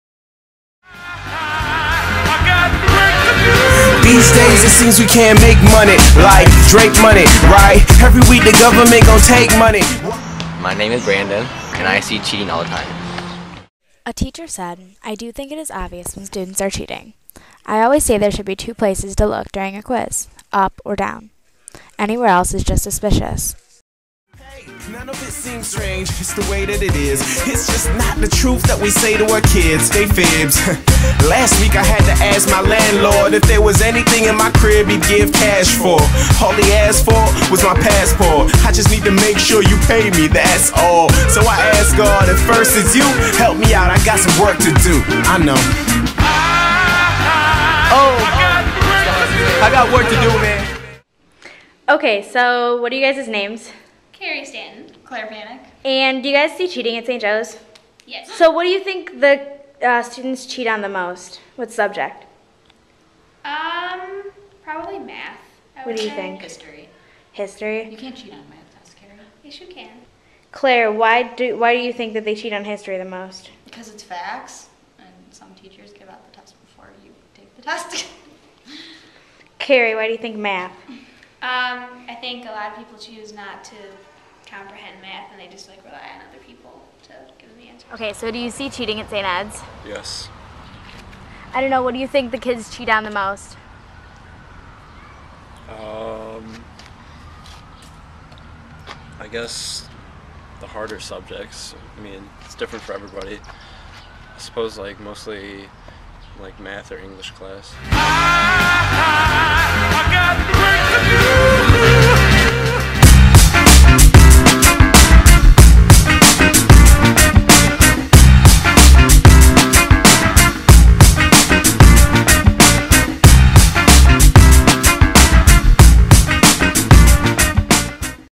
Cheating - Final Project
Haley's final project about cheating in school for the Spring 2011 MyMedia class